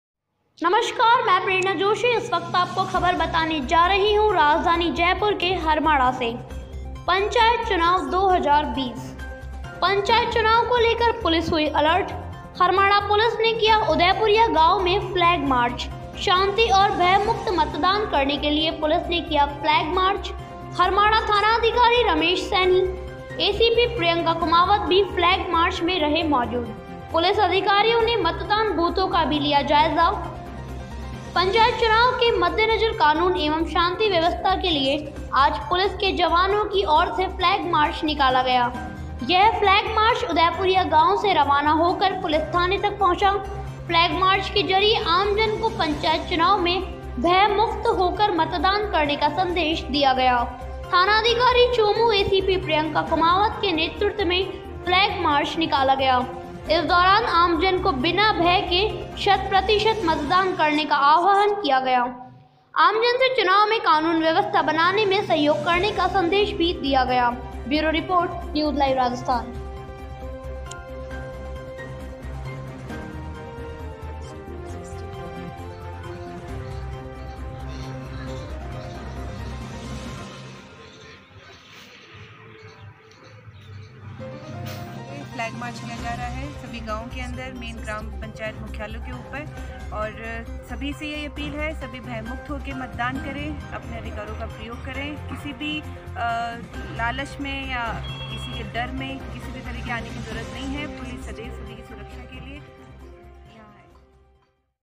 News Live Rajasthan आमजन को पंचायत चुनाव में भयमुक्त होकर मतदान करने का संदेश दिया, चौमू एसीपी प्रिय
News Live Rajasthan आमजन को पंचायत चुनाव में भयमुक्त होकर मतदान करने का संदेश दिया, चौमू एसीपी प्रियंका कुमावत के नेतृत्व मैं निकाला फ्लैग मार्च